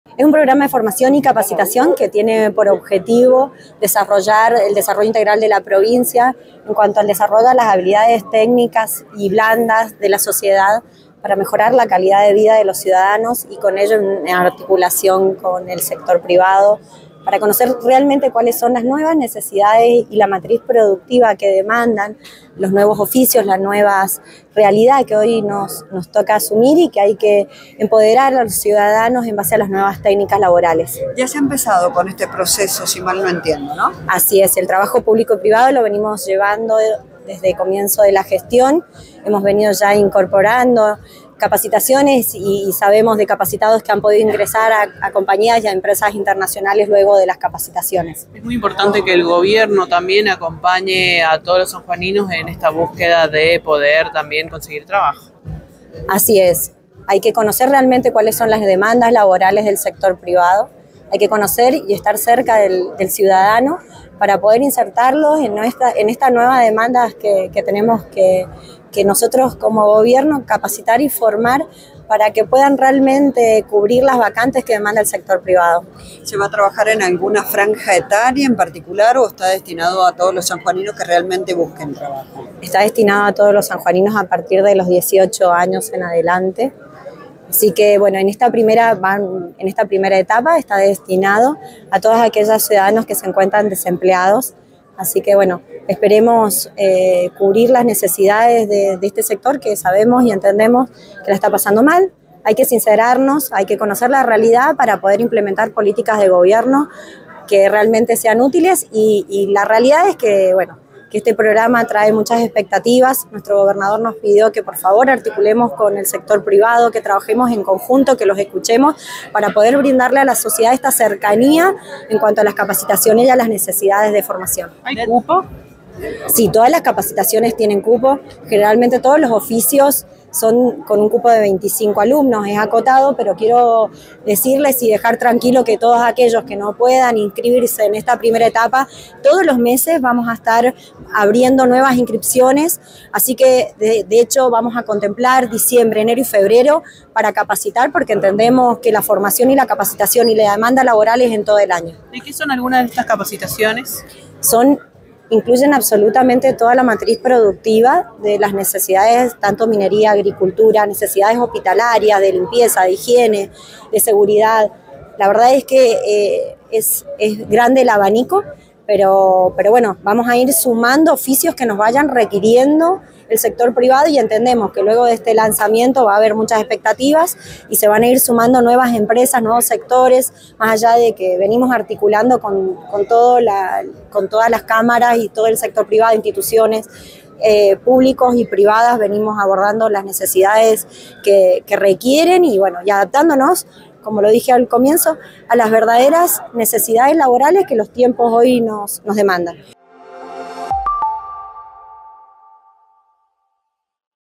Programa de Empleo, en una primera etapa, será para personas que no estén trabajando o informal
Luciana Cuk, directora de empleo explica como se desarrollara el programa. Se realizará inscripciones todos los meses